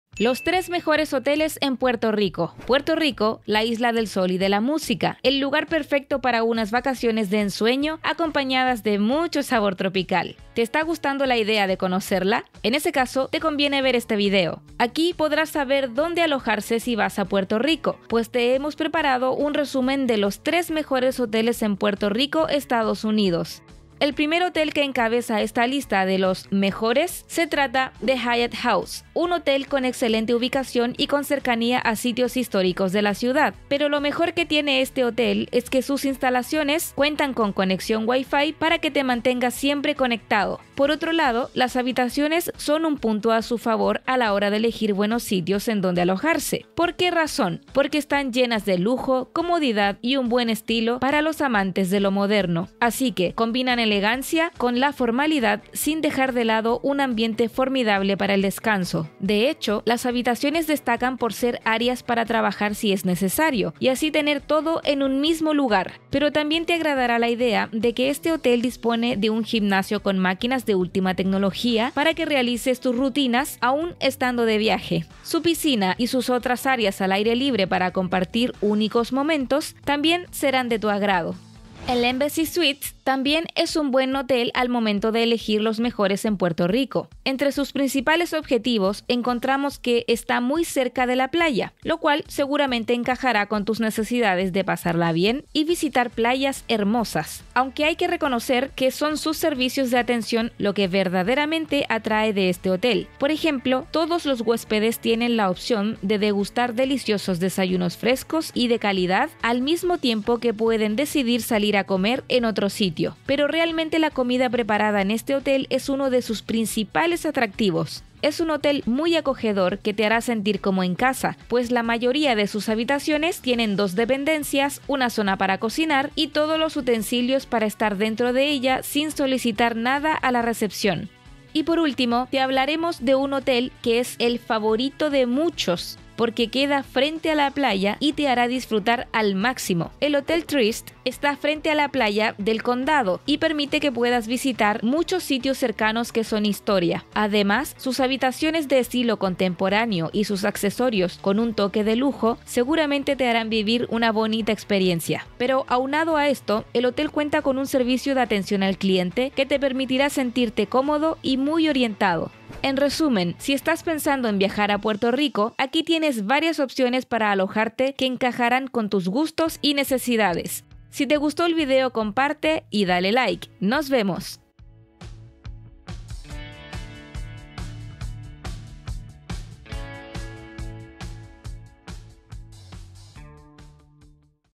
🥇 Los 3 Mejores Hoteles en Puerto Rico (USA) en 2025
¿Cuáles son los mejores hoteles de Puerto Rico en relación calidad-precio? Descúbrelo en este vídeo.

NOTA LEGAL